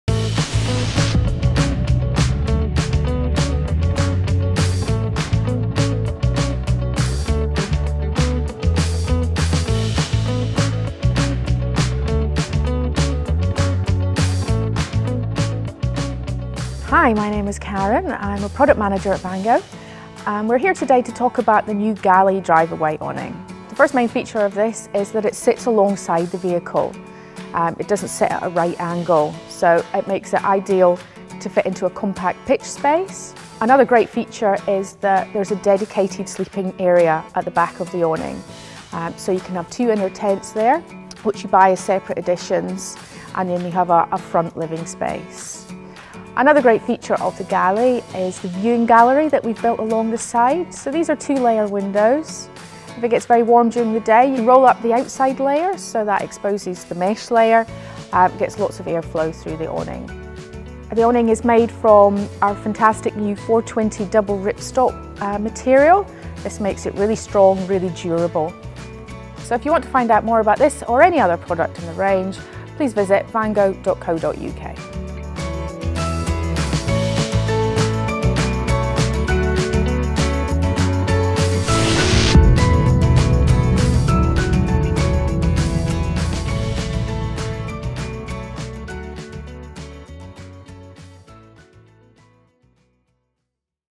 Vango Drive-Away Awning - Galli AirAway® filmed 2016
Three words express the very essence of the new Galli: innovative, contemporary and versatile. It’s impressive in structure and design, ideal for anyone who wants a larger and more usable space from their awning. The unique pre-bend AirBeams and curved roof offers greater comfort and room to live in. There is even the option to add in one or two inner tents to offer dedicated sleeping space at the rear. Constructed with Vango Sentinel Pro 420- a high quality double ripstop polyester fabric for superior strength and long lasting durability. Flexibility is key in a drive away inflatable awning and to enhance this, we have included a side door in the connecting section between the awning and your vehicle, creating a great space for storage that will help your space clean and dry. The Galli brings a fresh approach to drive away awnings.